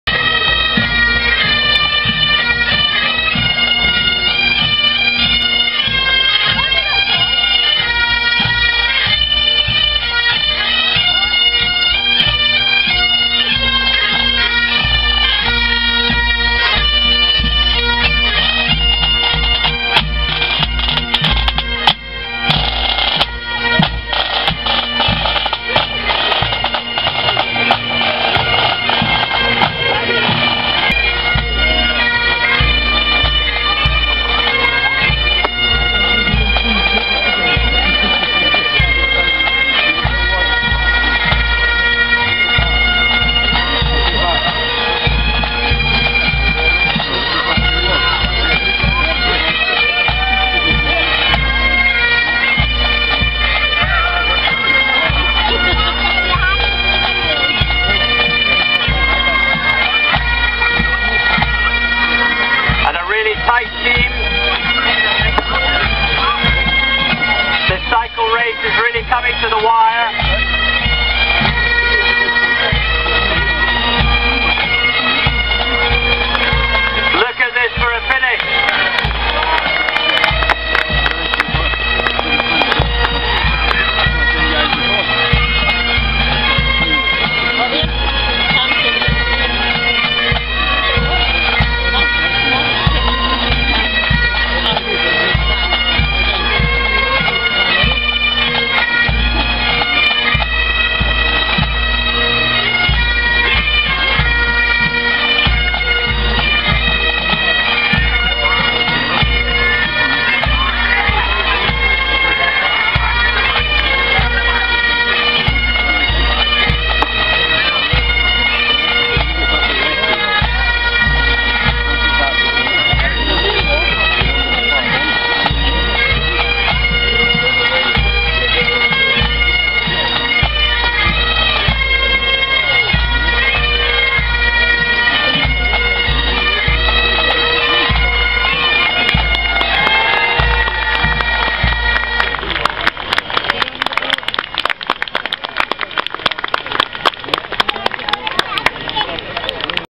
Northern Constabulary Pipe Band 2013
Northern Constabulary Community Pipe Band from Inverness are the Grade 4 Pipe Band lead by Pipe Major Kenny Watson.

Formed in 2002 and the Tartan is the Scottish National is the police service for the Highland Region & Islands few members are locals, retired staff, supports, family and friends of pipers and drummers.